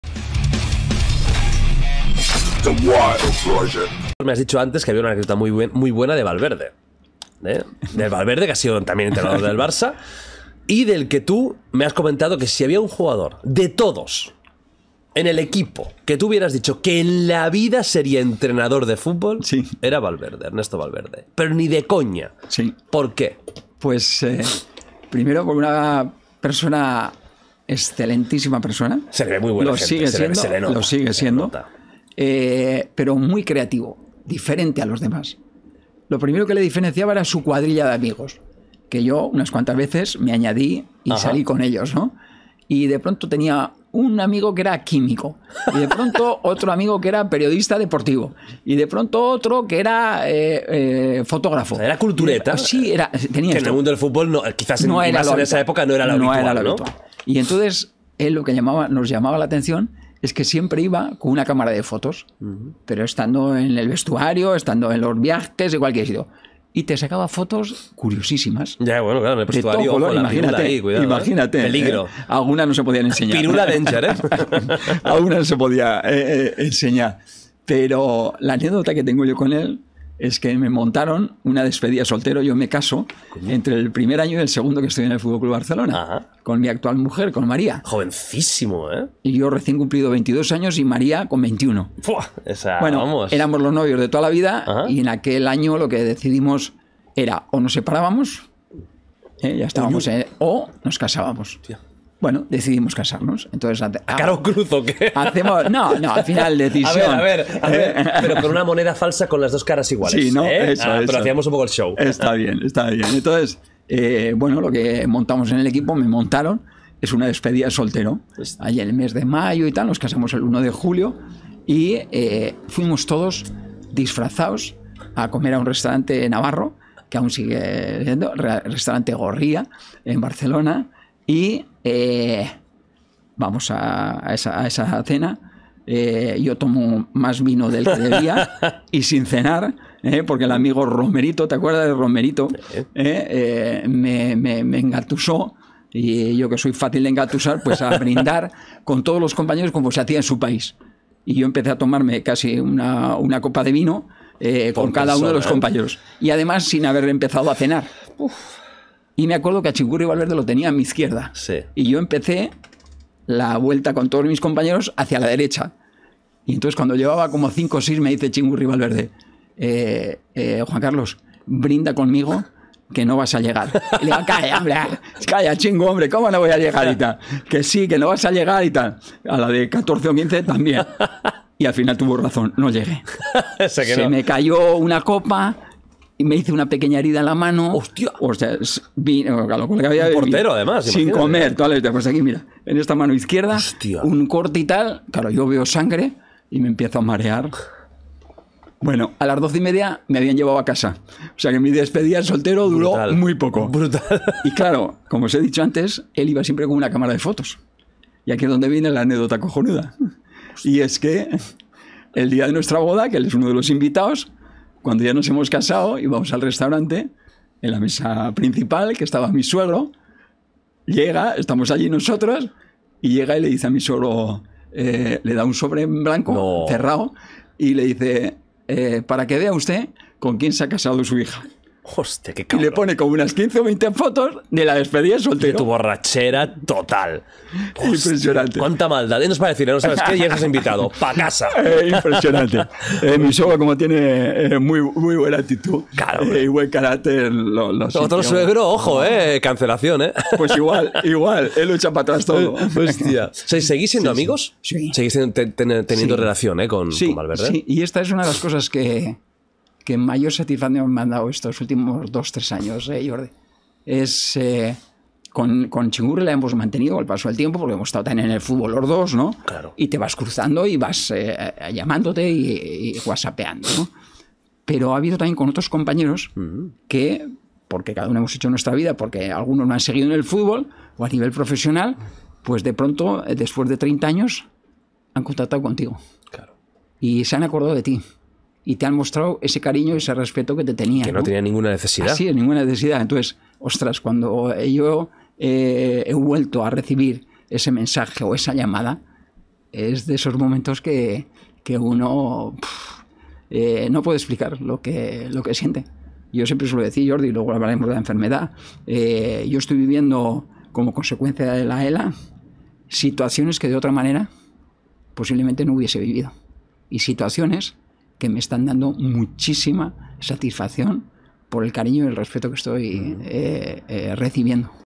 "NUNCA hubiera dicho que Ernesto Valverde sería entrenador" - Juan Carlos Unzué sobre el Txingurri
Juan Carlos Unzué, ex-portero de Primera División, entrenador junto a Luis Enrique de un Barça histórico (Messi, Piqué, Iniesta, Xavi, Neymar...) ganador de todos los títulos, anunció en 2020 que padecía ELA, la temible Esclerosis Lateral Amiotrófica, una enfermedad neurodegenerativa que avanza rápido y que le acabará postrando en una cama sin capacidad de moverse ni hablar, dependiendo de asistencia y máquinas para vivir. Juan Carlos, aún y con eso, tiene una actitud positiva, luchadora y optimista, y es una excelente fuente de energía para todos aquellos que lo están pasando mal. No te puedes perder este episodio, porque cambiará tu forma de ver muchas cosas.

- El nuevo libro de Jordi Wild